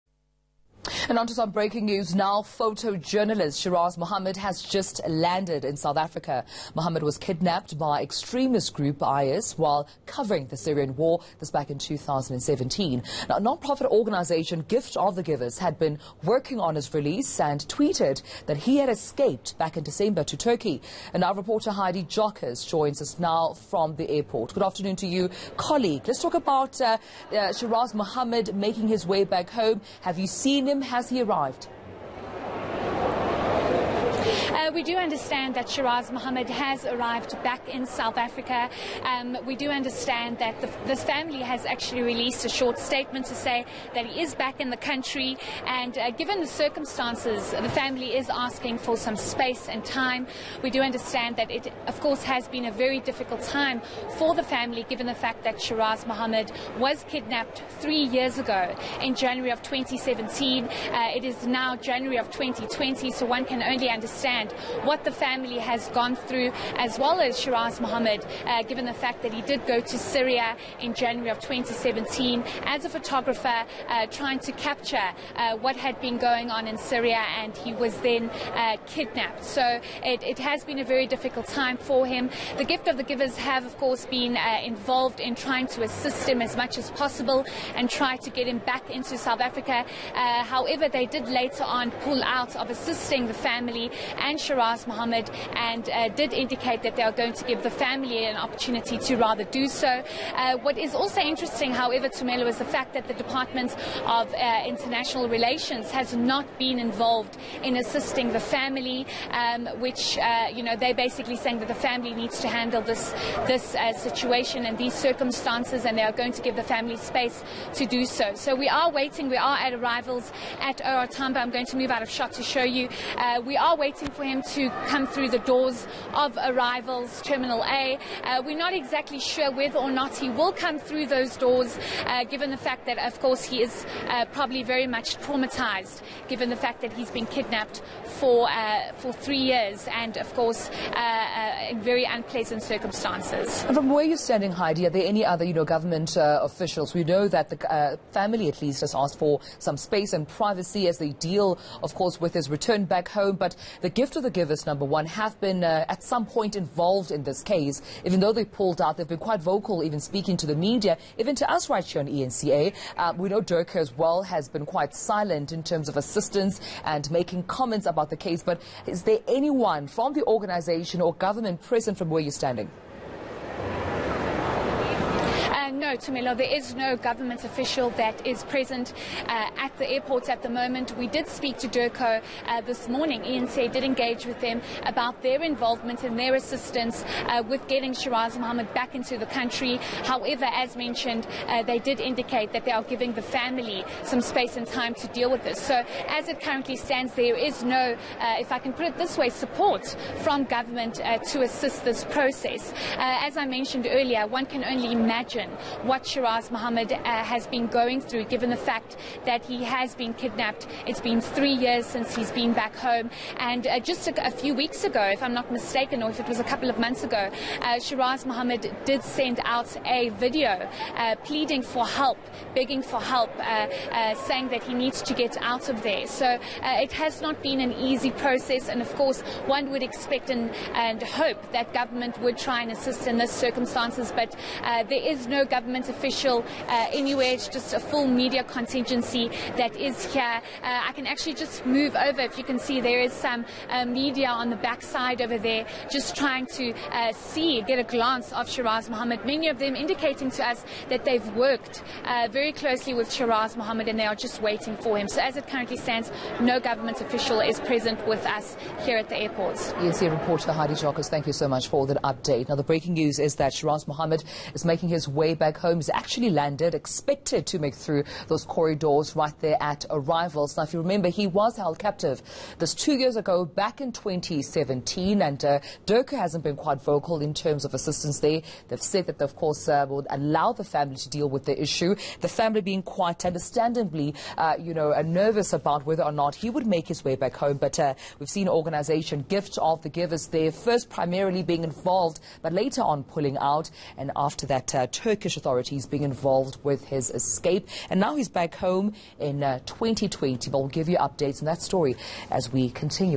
Shiraaz Mohamed has returned home
Photojournalist Shiraaz Mohamed has returned home from Syria after being held in captivity since 2017. Courtesy